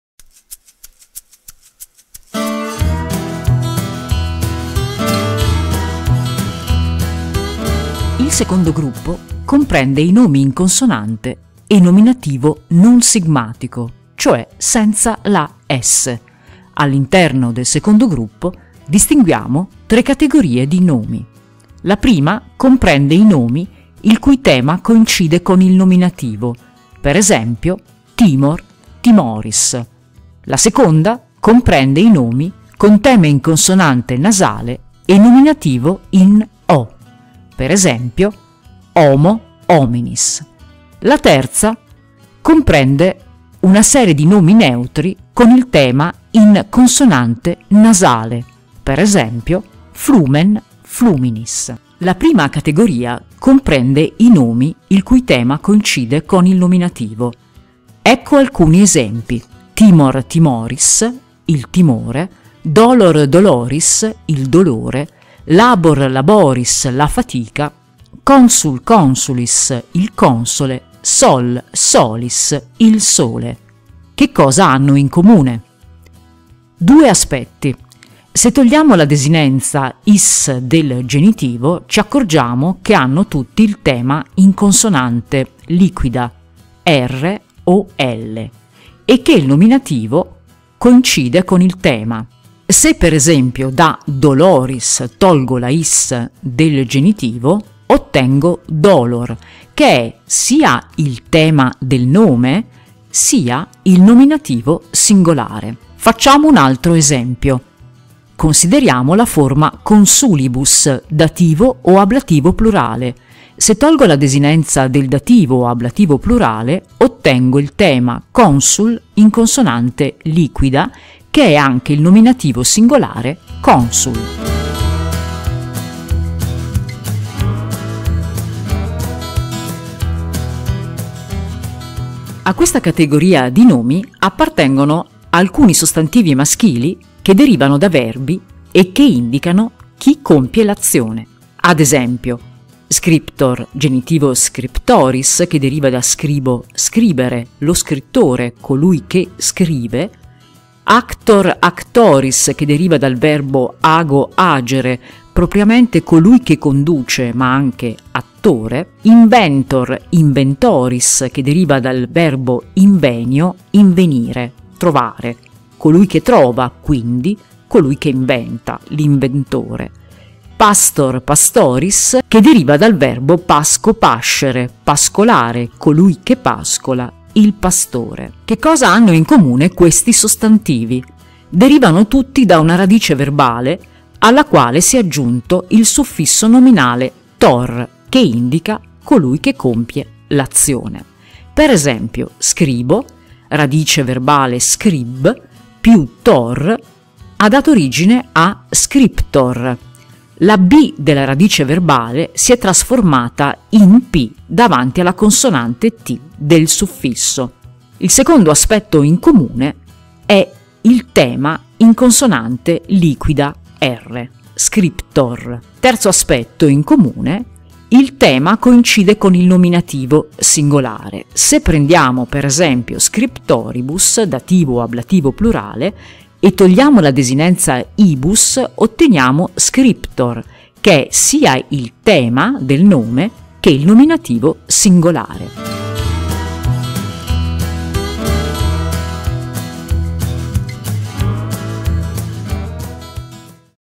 Terza declinazione: il secondo gruppo (prima parte)
Nomi con il tema in consonante liquida (-or) e nomina agentis (es. actor, actoris)